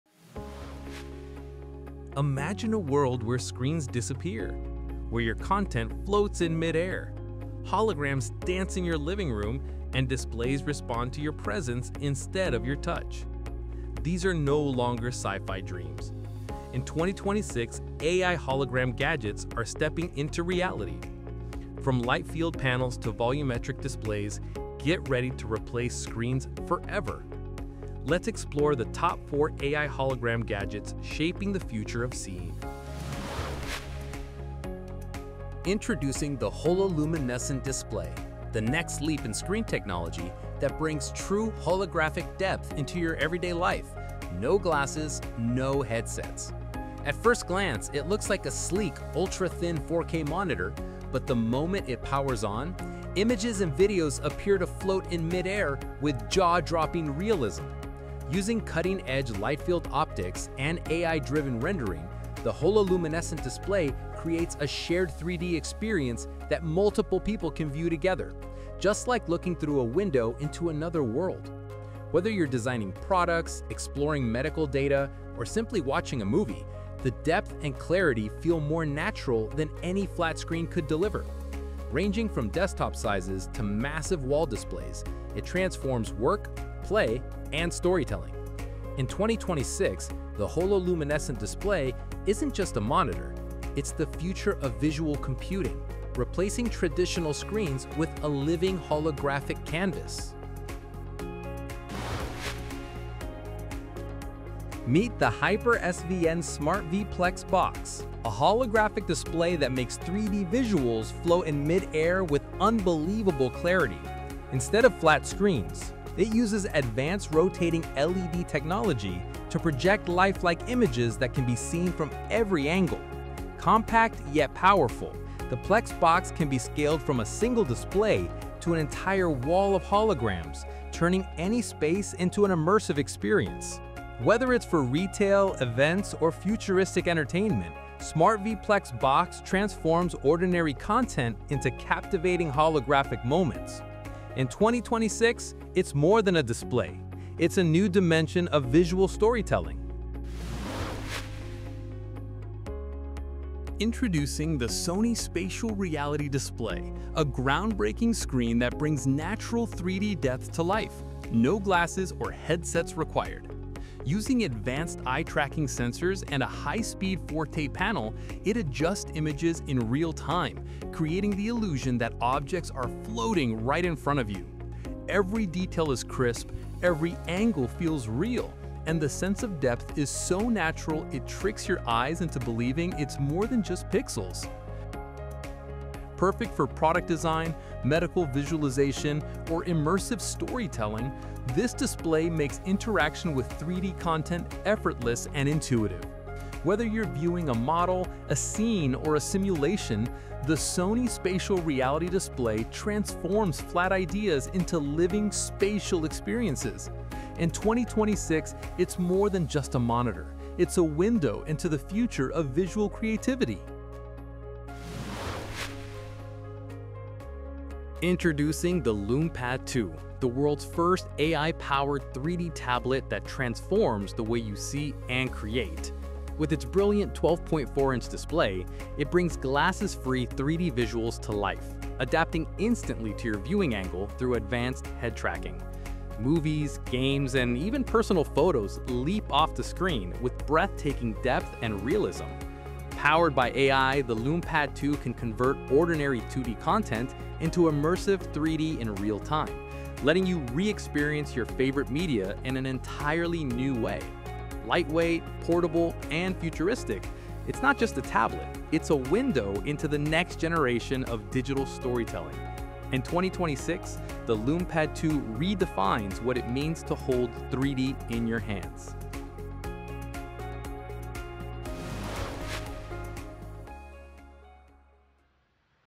Top 4 AI Hologram Gadgets of 2026 That Replace Screens Forever
Welcome to Make Life Smart! 🌌
In 2026, displays aren’t just getting bigger—they’re disappearing. From light-field panels to volumetric “floating” visuals, AI hologram tech is bringing 3D content into mid-air with no glasses or headsets. These breakthrough gadgets turn work, movies, calls, and design into living holograms you can view from multiple angles—like looking through a window into another world.

Products Links and Details:
1). 00:36 - Hololuminescent™ Display
2). 01:44 - HYPERVSN SmartV Plex Box
3). 02:33 - Sony Spatial Reality Display
4). 03:37 - Lume Pad 2

✨ Watch till the end to see how holograms are set to replace traditional screens forever.